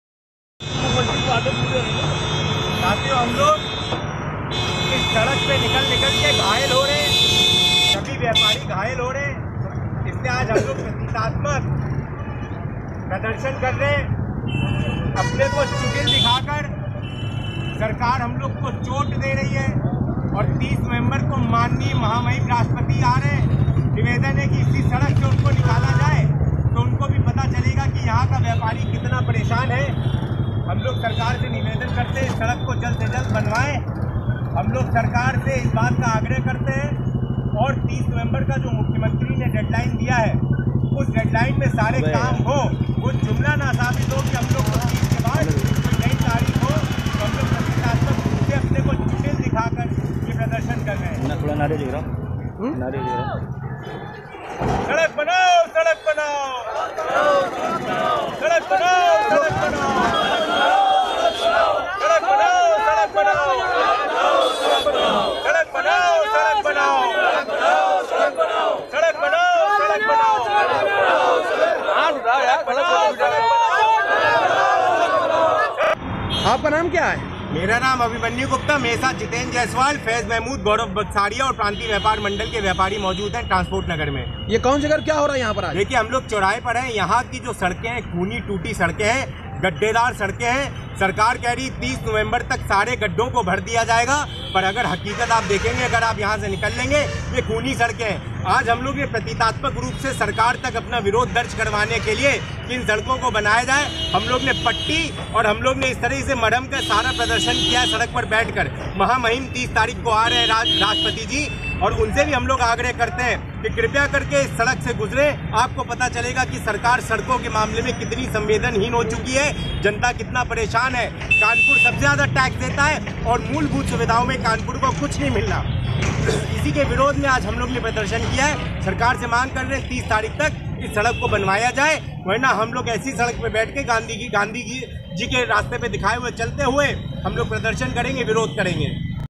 व्यपारियो ने प्रदेश सरकार के खिलाफ किया प्रदर्शन
public akrosh news

कानपुर:-टूटी सड़को के खिलाफ व्यपारियो ने प्रदेश सरकार के खिलाफ किया प्रदर्शन।
प्रतीतात्मक चोट की खून के रंग का मरहम पट्टी,बैंडेज लगाकर गांधीवादी अंदाज़ में किया प्रदर्शन।

टूटी खूनी सड़कों ने बहुत दी चोट,
सड़क के नाम पर नहीं मिलेगा वोट"।

30 नवम्बर जाएगी नया जुमला लाएगी " का नारा किया बुलंद ।
आज उत्तर प्रदेश प्रान्तीय व्यापार मण्डल के व्यापारियों ने टीपी नगर में किया प्रदर्शन।
ढाई साल बीत जाने के बाद भी सड़क गड्ढा मुक्त तो नही हुई बल्कि गड्ढा युक्त जरूर हो गयी।